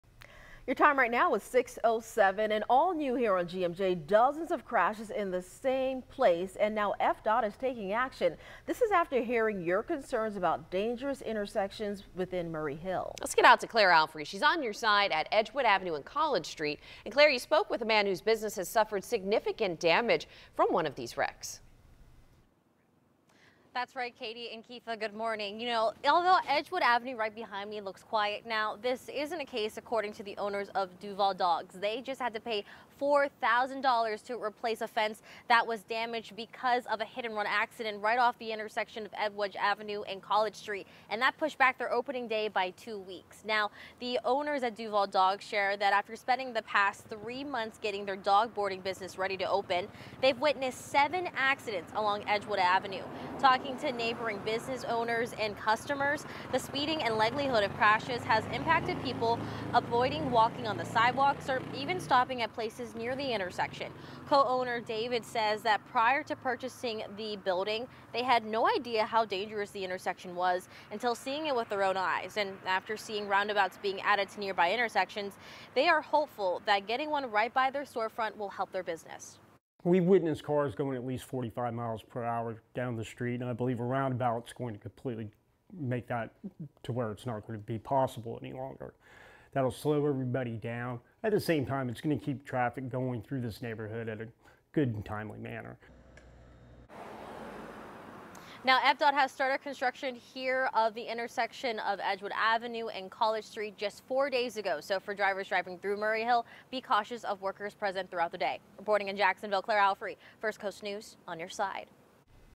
FDOT adding roundabout at intersection in Jacksonville's Murray Hill area after dozens of crashes
Construction on the roundabout at Edgewood Avenue and College Street started Saturday.